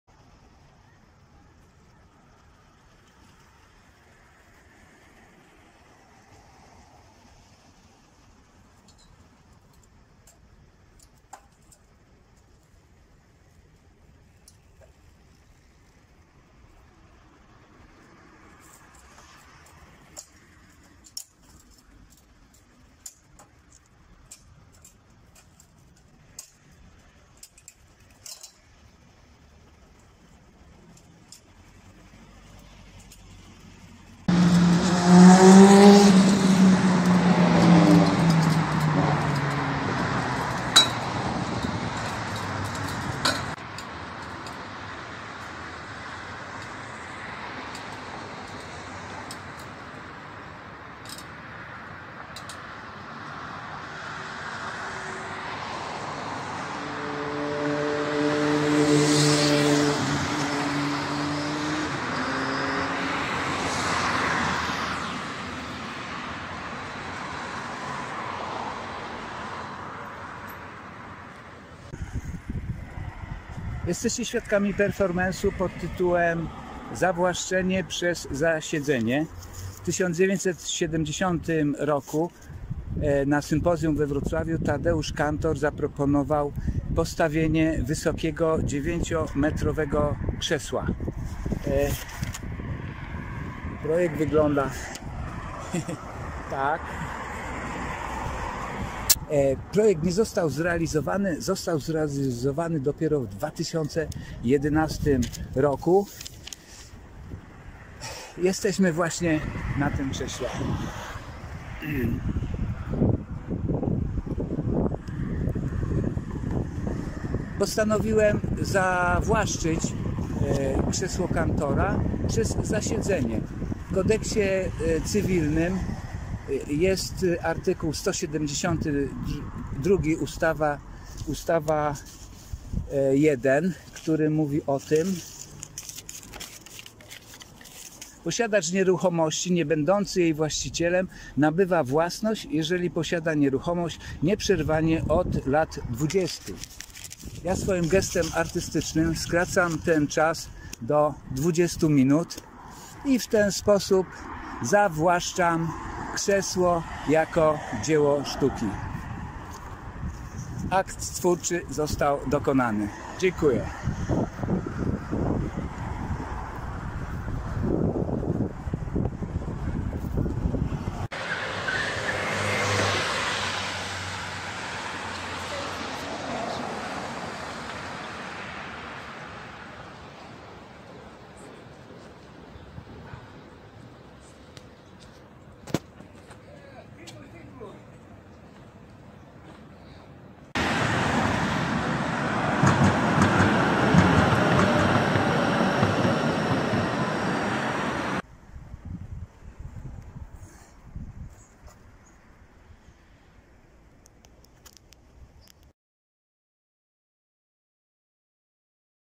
Jerzy Kosałka | KRZESŁO KANTORA | performance
Na wrocławskim Sympozjum Plastycznym w 1970 roku Tadeusz Kantor zaproponował postawienie w przestrzeni publicznej dużego betonowego krzesła o wysokości 9 m., co zrealizowano dopiero w 2011 roku. Dwa lata później, podczas remontu jezdni zdemontowano tabliczkę z podpisem tej rzeźby.
Projekt polega na artystycznym zawłaszczeniu krzesła przez zasiedzenie. W polskim Kodeksie Cywilnym, w rozdziale II, art. 172, par. 1, istnieje zapis: Posiadacz nieruchomości nie będący jej właścicielem nabywa własność, jeżeli posiada nieruchomość nieprzerwanie od lat dwudziestu przez zasiedzenie. Podczas performensu, własnym gestem artystycznym, artysta skrócił 20 lat do 20 minut, siedząc przez ten czas na krześle. Schodząc przyczepił do krzesła tabliczkę z własnym nazwiskiem. W ten sposób nastąpiło artystyczne zawłaszczenie dzieła Kantora.